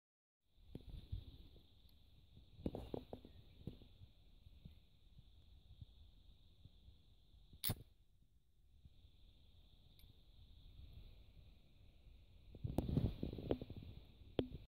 Pedersoli French AN IX misfire
First time out shooting the Pedersoli French AN IX (year nine) .69 caliber pistol. At this point, I had fired the pistol about 7 times with no issues, swabbing the barrel every two shots. Had a misfire, primed the pan again and it went off fine.